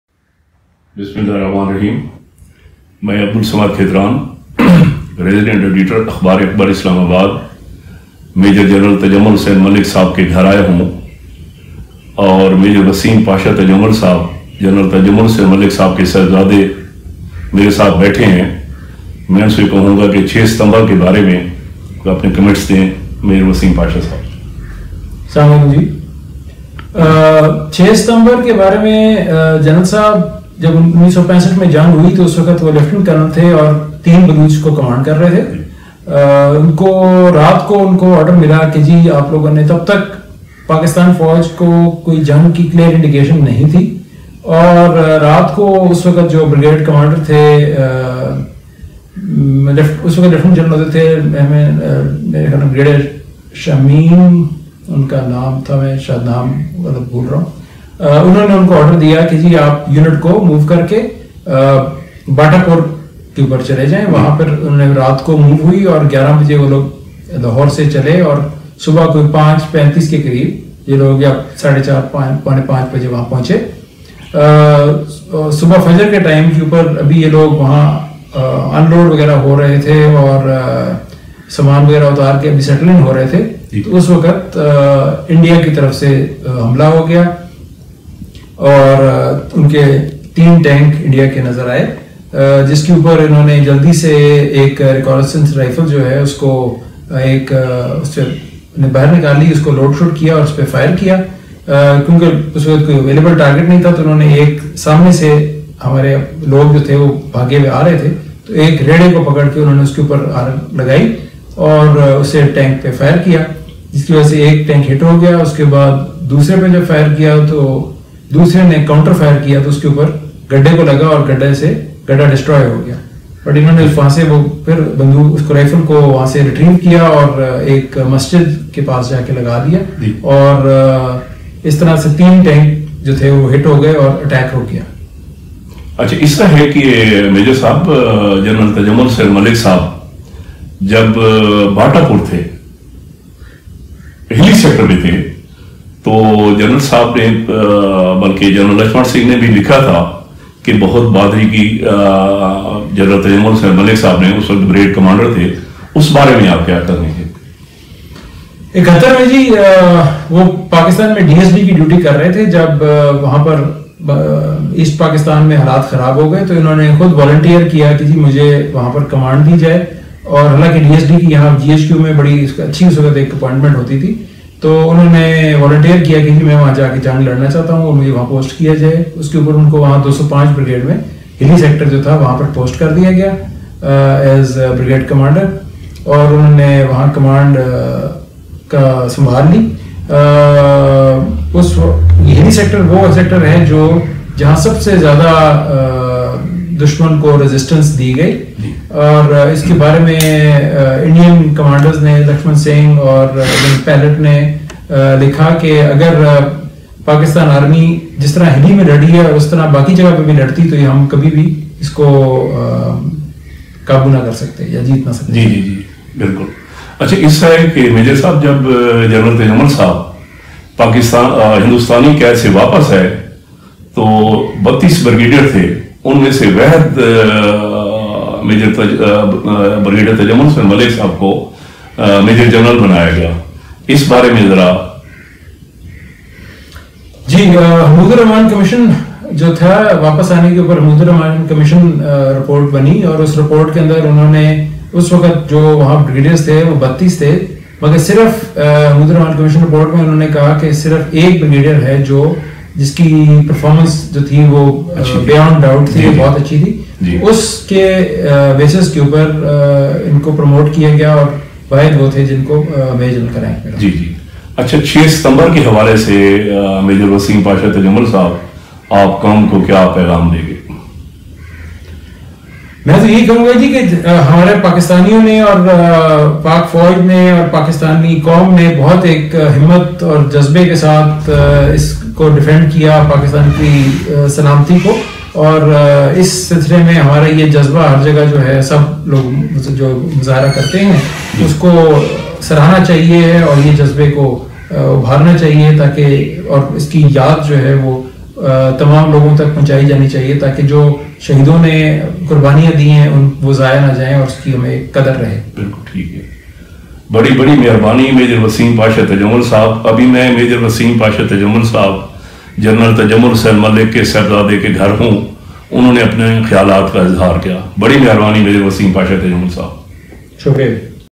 The story of Major General Tajammul Hussain Malik oral of his son Major Waseem Pasha Tajamal
Tajammul Hussain Malik, from the Awan tribe, was born on June 13, 1924 in the Chakwal District in the Punjab province and became a career officer in the British Indian Army, 7 Rajput Regiment, later joining the army of the new state of Pakistan in 1947.[6] He had participated in Indo-Pakistani War of 1947 and the 1965 War-Battle of Batapur. In 1969, he was promoted as Brigadier and in October 1971 he was posted as DSD[clarification needed] in GHQ, from where on his personal request to volunteer and fight in East Pakistan was posted to 205 Brigade deployed on the Hilli-Bogra Front and joined the Brigade on November 20, 1971 deployed at Hilli in East Pakistan (now Bangladesh).[7]

During the war, he was cited for a live award of Nishan e Haider (the highest award for valour in the Pakistani Armed Forces);[citation needed] Malik's unit continued fighting even after Pakistani forces formally surrendered to Indian forces in Dhaka.[7] He was the Commander of the Pakistani Forces in the Battle of Hilli He was the only red tape Pakistani officer who did not surrender while his GOC 16 Division had surrendered and as he had been taken from the battle field unconscious he refused to undergo the surrender ceremony and it was his GOC. Major General Nazar Hussain Shah, who went through the surrender ceremony of 205 Brigade to the opposing commander Major General Lachhman Singh.[7] He became a well known war hero in West Pakistan.